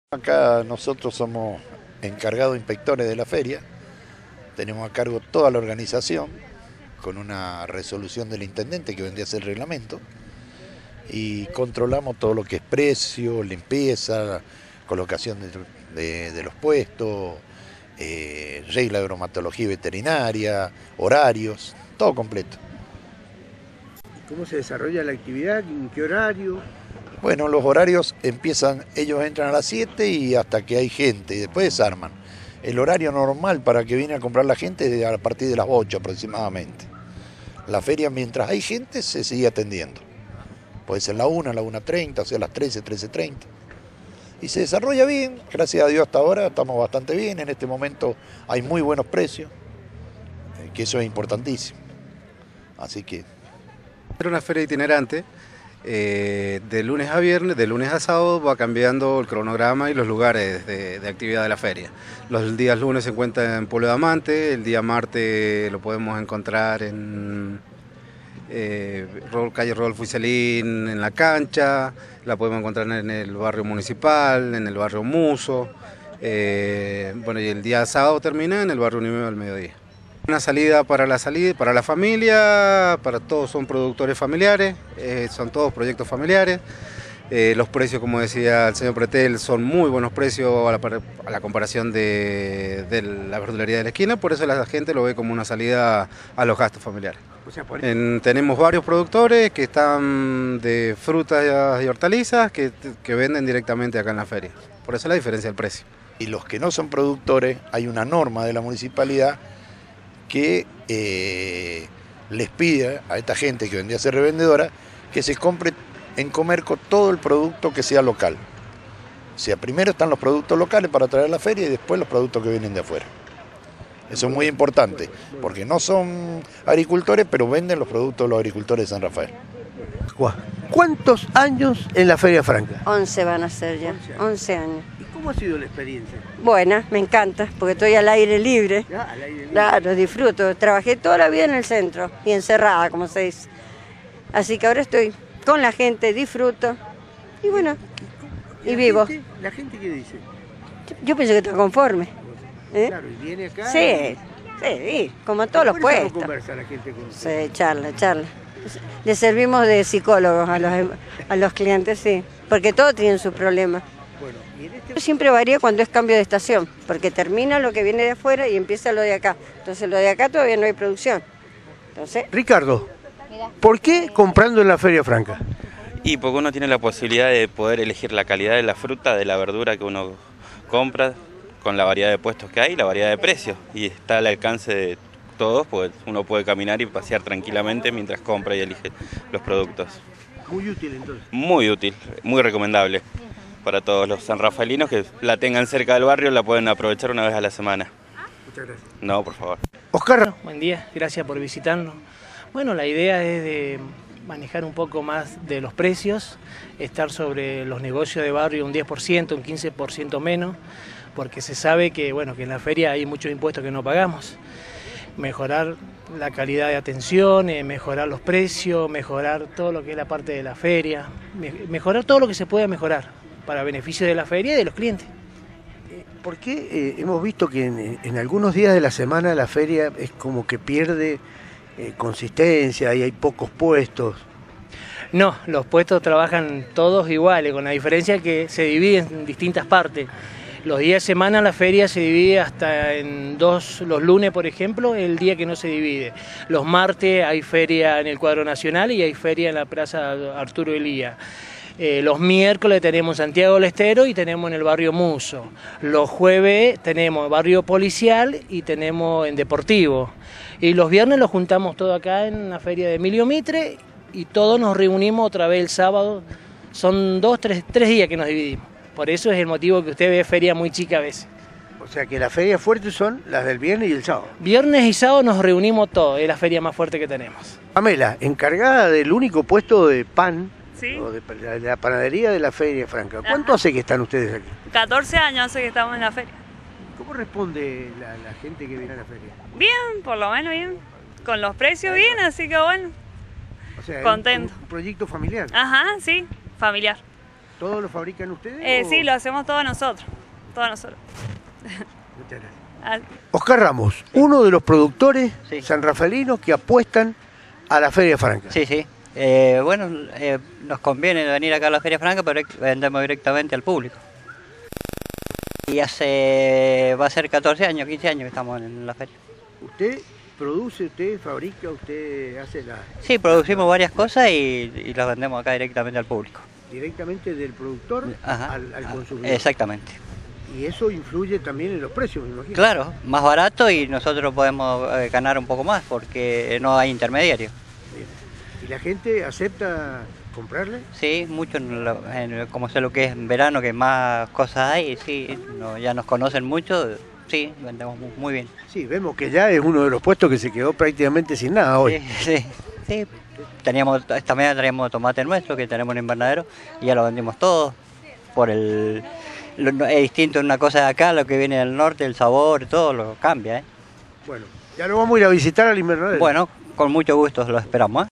Informe de Cana 10 TV San RAfael sobre Feria Franca Municipal
CN10TV y Canal 10 Libres San Rafael Mendoza entrevista a Encargados, Feriantes, Productores y compradores sobre aspectos importantes del funcionamiento de la Feria Franca Municipal de San Rafael Mendoza